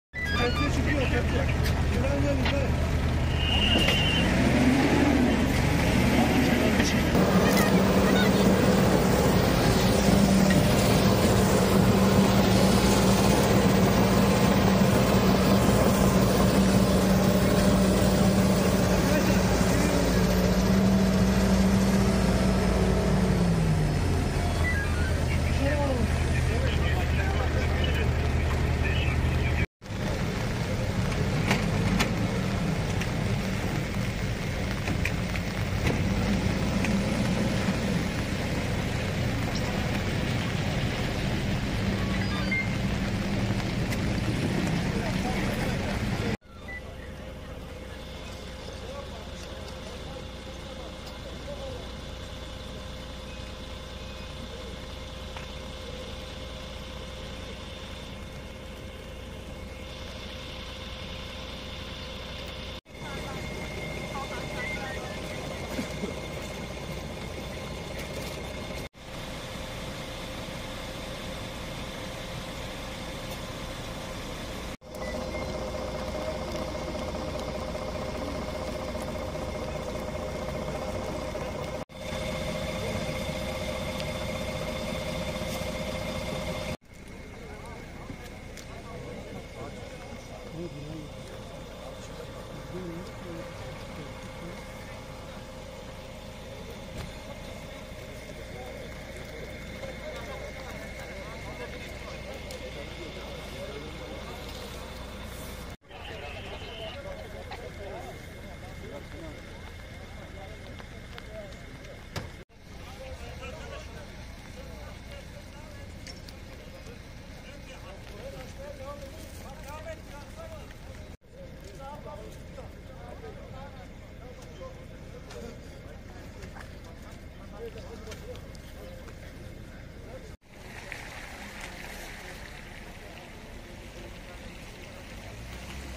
Li Meletîyê avahî di dema xerakirinê de hilweşîya: Kesek di bin xirbeyan de ye
Di encama rûxandina avahîyeke ku ji ber erdheja Meletîyê zirareke mezin dît, di dema hilweşandina bi kontrol de kesek tevî makîneya kar a ku bi kar dianî di bin xirbeyan de ma.

بینەرانی بەڕێز؛